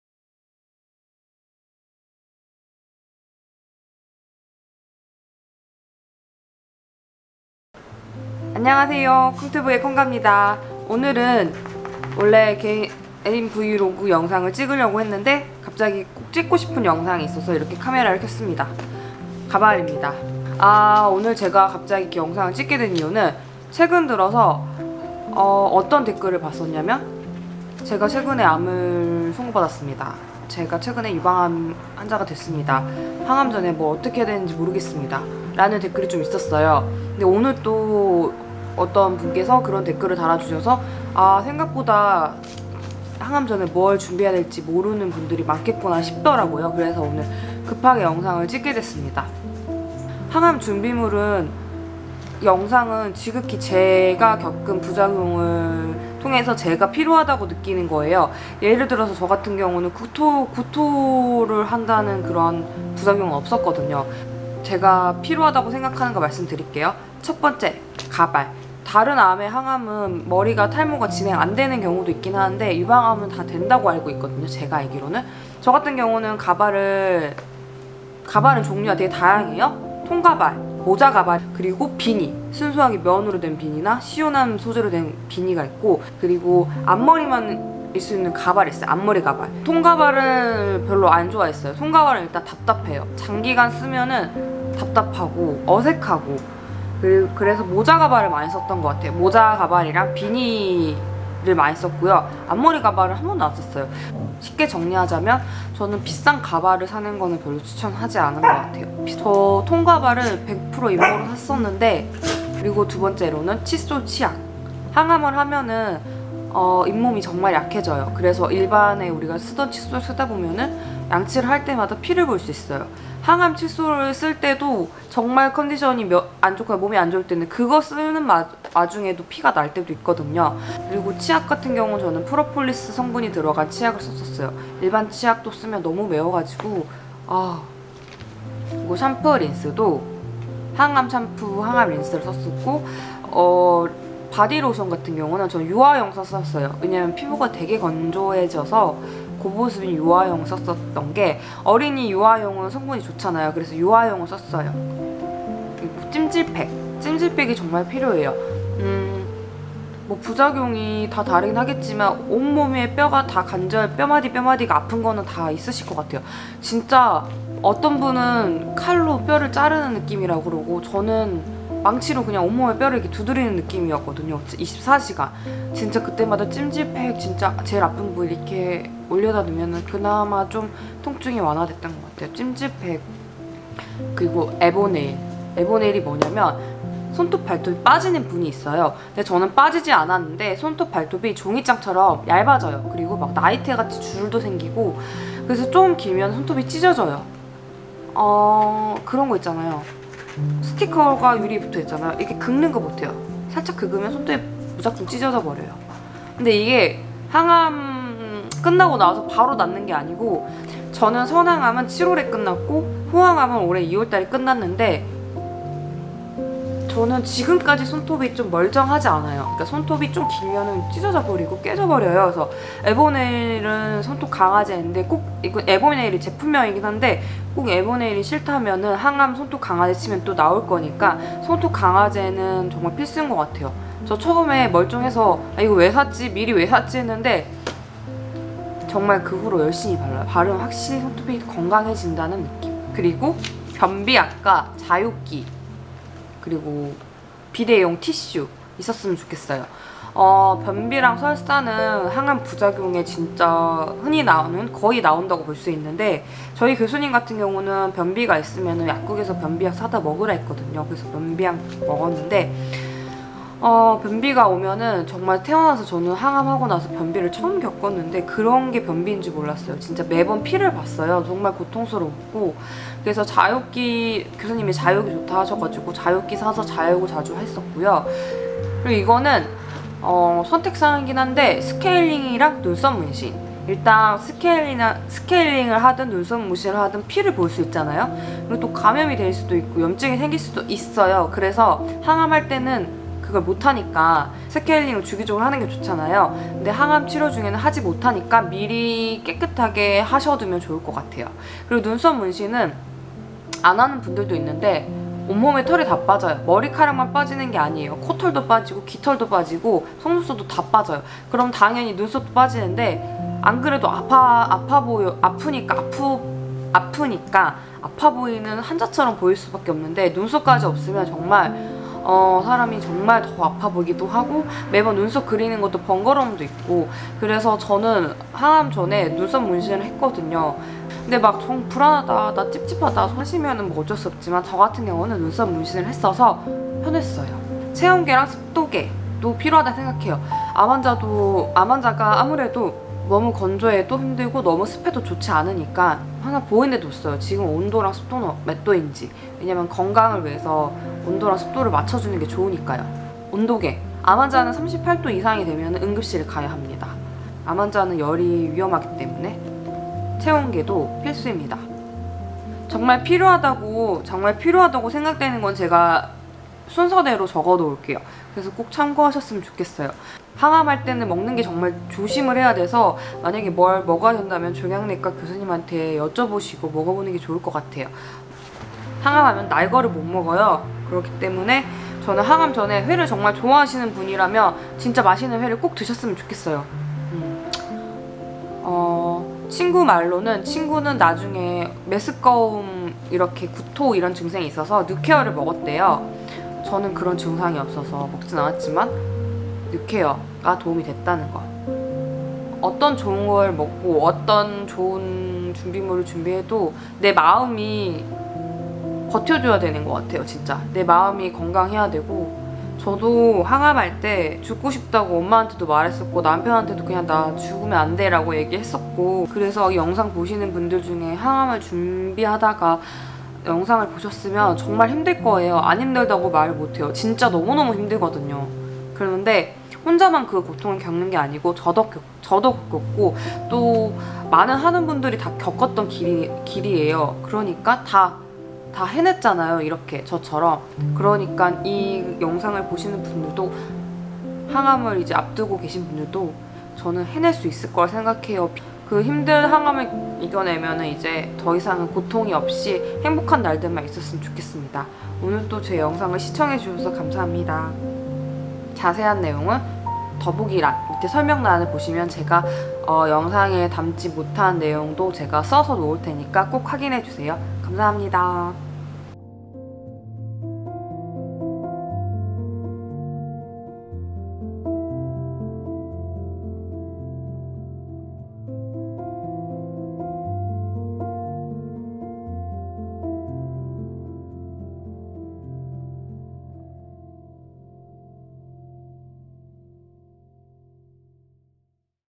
항암 준비물 | 항암 하기전 해야하는 것 | 4기 유방암 환자가 직접 겪은 경험담
안녕하세요.
제가 가발 쓰면 예뻐보일지 알고 쓰고 찍었는데
뭔가 애호박같이 생겼네요...😰
앞으로 이 가발 쓰고 영상 찍지 않을거랍니다.. 양해 바랍니다.

제가 생각하는 항암준비물 우선순위
1.가발
가발은 저는 개인적으로 ! 너무 비싼건 추천드리지 않습니다.
적당한 가격선에서 다양한 가발을 쓰시는게 좋을거 같아요.
저도 통가발 비싼거 샀는데 별루 쓰지 않았어요.
저는 모자가발과 두건 비니를 주로 썼어요.

2.좌욕기
좌욕 정말 자주하세요. 꼭 변비가 아니더라도 좌욕은 좋습니다
습관화 되면 좋을것 같아요

3.에보네일
항암 전부터 발라 주시고, 항암 중에도
그리고 항암 후에도 꾸준히 발라주시면 좋아요

4.찜질팩
찜질팩은 정말 필수입니다. 꼭 !!
온몸이 아팠거든요.

5.칫솔,치약,샴푸,린스,바디로션
저는 테스칫솔, 치약은 프로폴리스 들어간 치약
샴푸, 린스는 항암용으로 성분 착한것
그리고 바디로션은 어린 유아용을 성분 순한것으로 썻어요.

6.눈썹문신,스케일링
스케일링은 꼭 하세요. 치간칫솔 같은것도 항암중에는
안쓰는게 낫거든요. 피나면 피가 계속 나요...

7.체온계,습도계
항암중에는 열이 38도 이상이 1시간 이상 지속될 시
응급실에 가야합니다. 저도 2번 그랬는데
1번은 응급실에 갔고 그 후에 교수님께서 저는 뉴라스타 라는
백혈구 주사를 맞아서 열 오르면 해열제를 먼저 먹어보라해서
그 후로는 안갔습니당 :)

이 외에도 말씀드리고 싶은게

눈썹문신은 저같은경우에 항암전에 하는거라 말하고
리터치를 못한다고 말하니 더 신경써서 해주시더라구요.🙂

또, 항암중에는 답답하고 더워도 KF94 착용을 필수로 해주시면 좋겠어요.
감염에 민감하고 면역력이 약하다보니 특별히 신경쓰셔야 할거같아요.
저는 사람 많은곳도 잘 가지 않았어요.
감기 같은거 옮는것도
항암할때는 조심해야하거든요.

그리고 항암을 시작하면 주변분들이 무슨 버섯이 좋다
무슨 즙이 좋다 하는데 항암중에는 모든 음식을 조심해야하니
드시지 말고 종양내과 교수님께 꼭 물어보세요.

이외에 구내염과 인후염 예방으로 저는 생리식염수를 매일 사용했구요.
프로폴리스 스프레이도 구강에 뿌려주면 좋은데 저는 인후염과 구내염이 오지 않아서
몇번 쓰지 않았습니다.

더 궁금하신 부분 있으시면 댓글 주시구요.